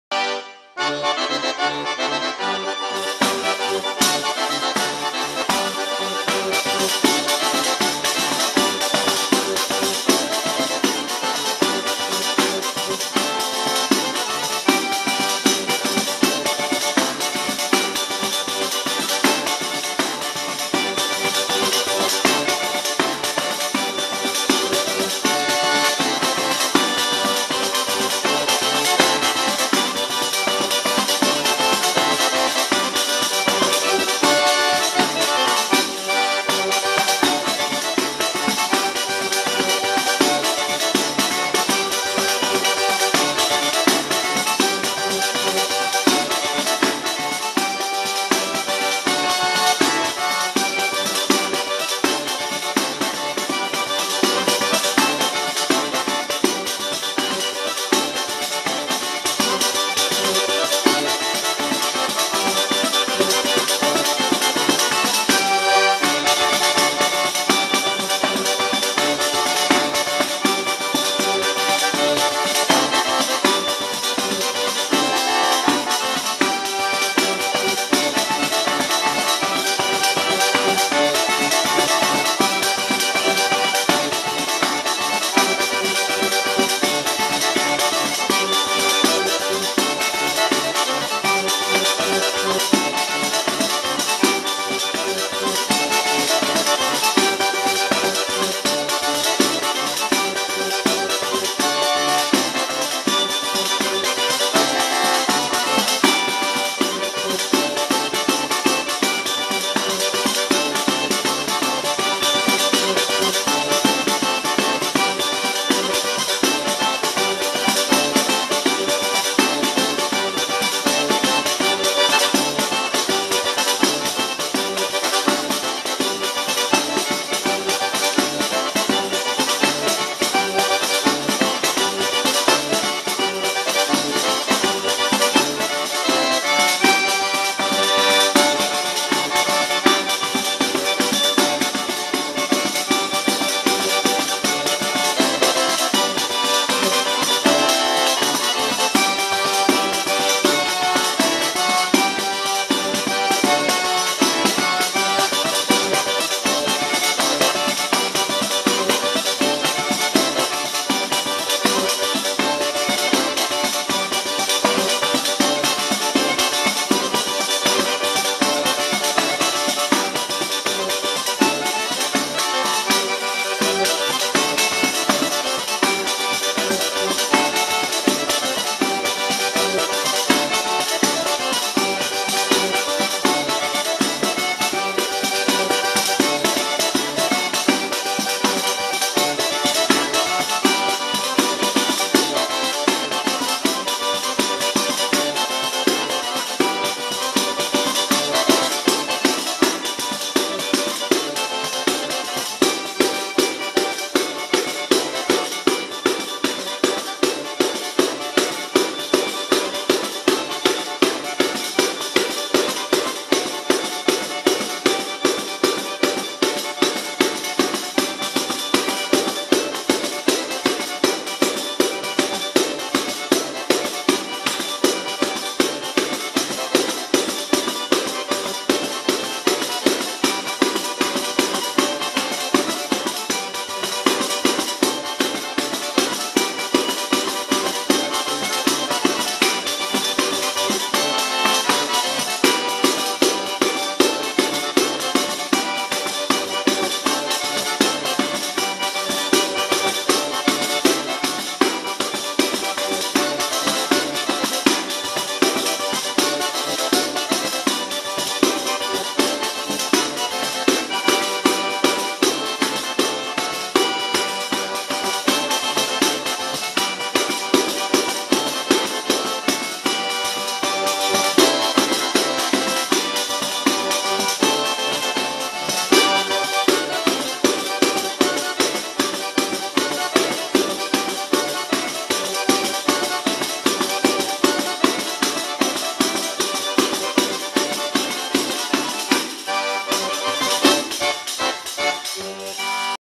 Tarantella con Sol ottavato Simone Filippone e Giuseppe Lucà
Suonata con organetto a sol ottavato binci modello classic edizione limitata.